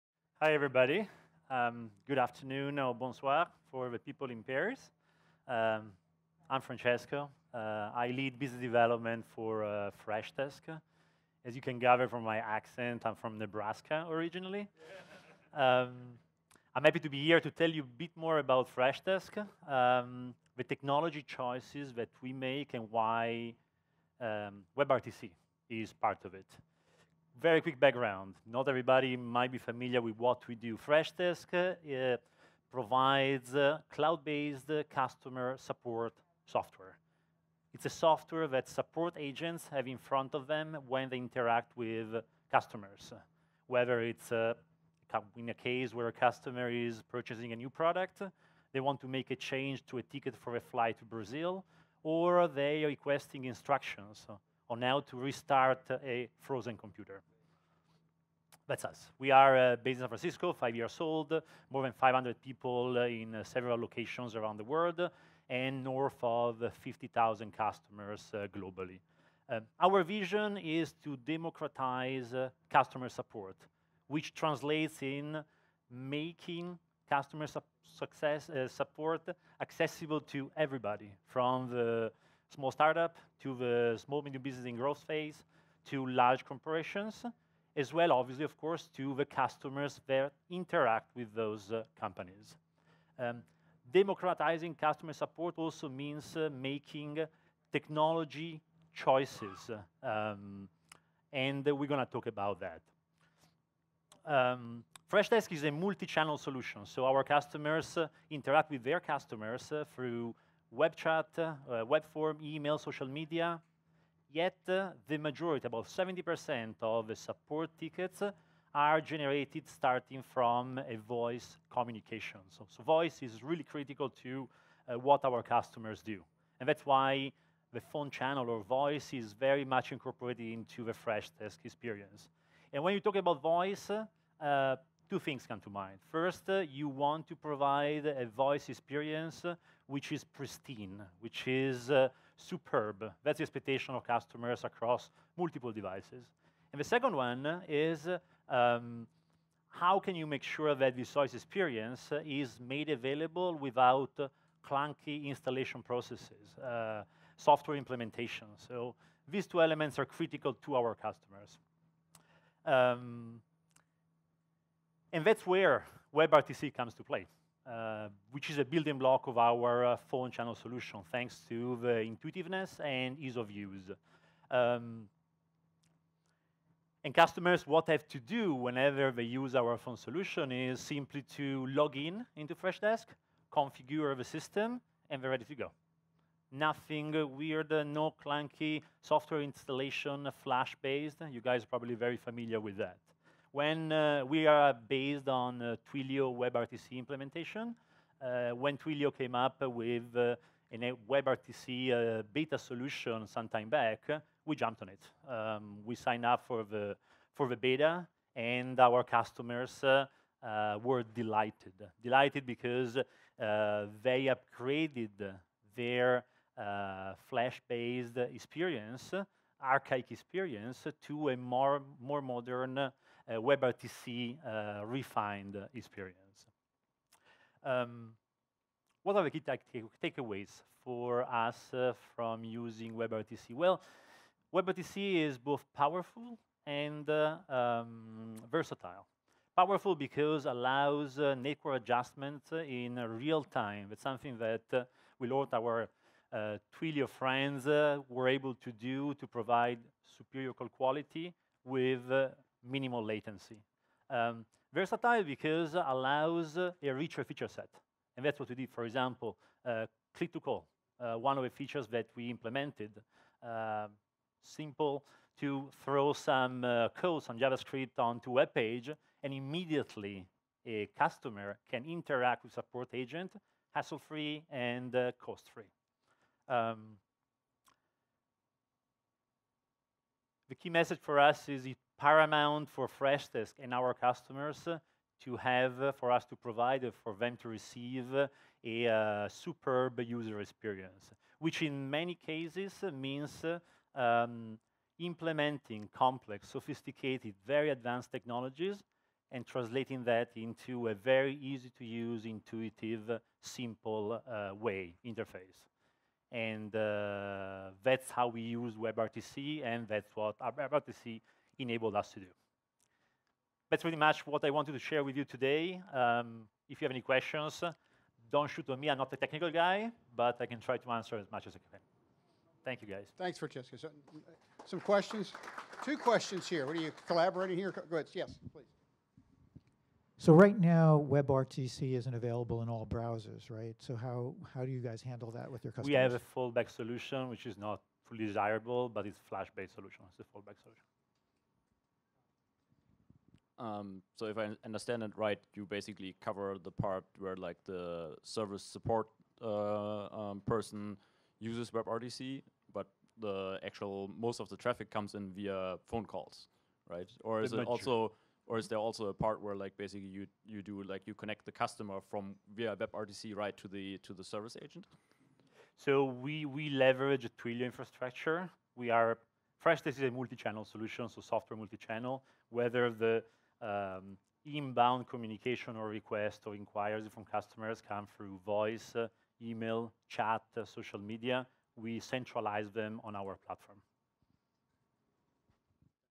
Application Chats: Freshdesk
Freshdesk has implemented near "call center" like capabilities as part of their customer experience management SaaS solution. This integration enables an "agent" to provide telephony support while utilizing the Freskdesk tools to solve the end users issue with no need for a traditional desktop telephone.